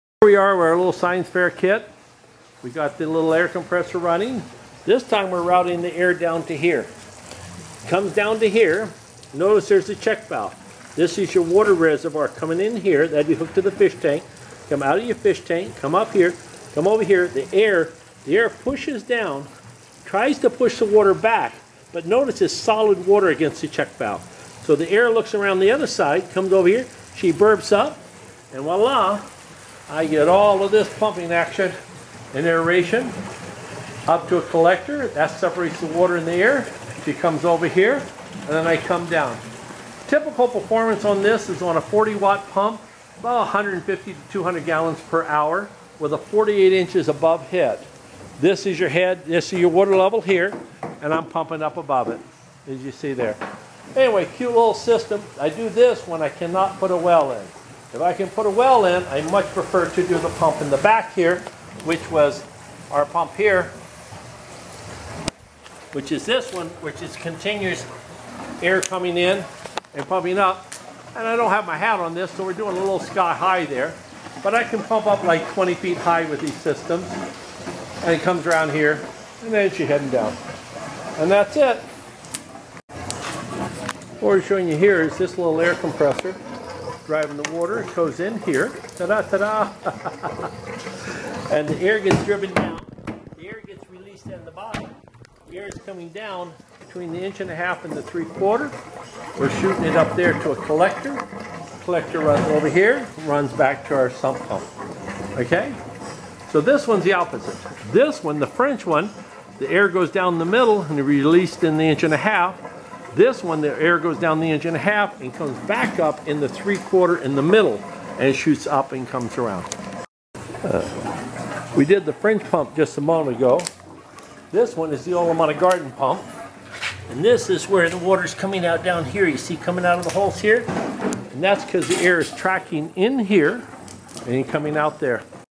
Olomana Gardens Educational Air Lift Pump Display
Glenn Martinez of Olomana Gardens did it again? You said what he did? We say having a demonstration unit that shows his different style of how he uses an air pump to move and lift water through our aquaponic systems here at Olomana Gardens. Pretty unique. Check it out and let us know what you think?